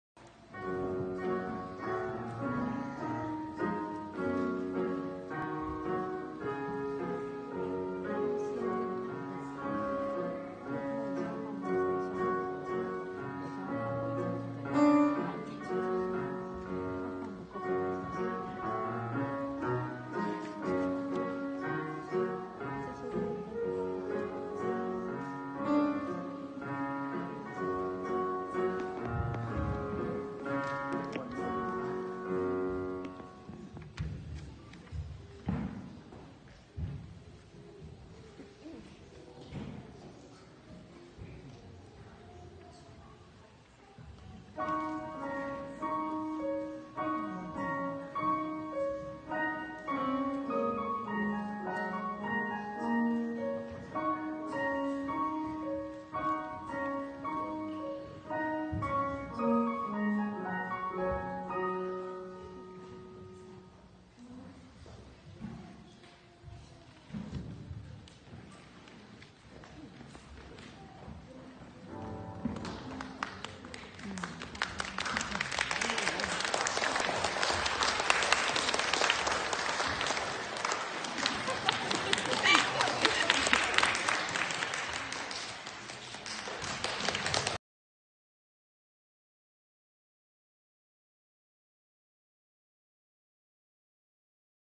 MuzikFun Education piano group class - annual recital
This performance is formed by two little piano group classes taught by Ms. Aimee performed on 11/17/2018 at Mt. Sac. Little performers age are from 4-6 years old!